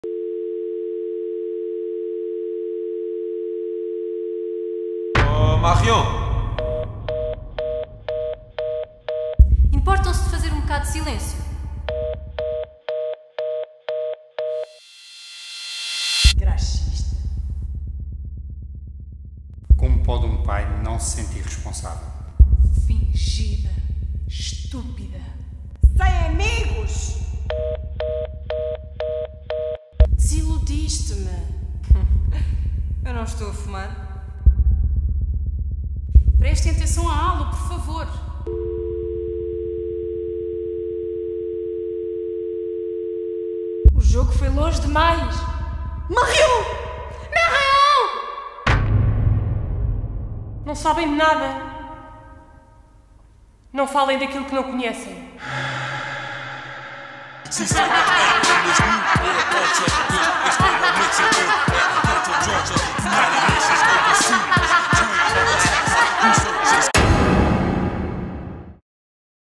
Teaser Forever Young
Forever Young

Sinopse:
O espetáculo Forever Young é baseado no livro “13 anos para sempre Marion” de Nora Fraisse. Um alerta comovente sobre um dos maiores perigos da nossa sociedade juvenil, o Bullying. Nesta proposta partimos do relato da mãe, do pai ou dos pais que procuram, mais do que culpados, respostas. Com base nos factos existentes, mergulhamos numa envolvente e perturbadora história. Neste relato emocionante procuramos desenhar os passos de Marion, um género de retrospetiva policial a partir das palavras deixadas pela própria, em cartas, mensagens, nas respostas aos ataques, as suas ações e comportamentos nas mediações da escola e circuitos juvenis envolvendo os potenciais agressores.

Dramaturgia, revisão de textos e direção de atores: Catarina Gonçalves e João Rosa
Encenação: João Rosa
Elenco: Andreia Águas, António Martins, António Silva, Carla Silva, Diana Fernandes, Dilma Almeida, Edite Oliveira, Frederica Cascais, Isabel Dias, Joana Martins, Leonor Lobo, Patrícia Barroca e Sílvia Sousa.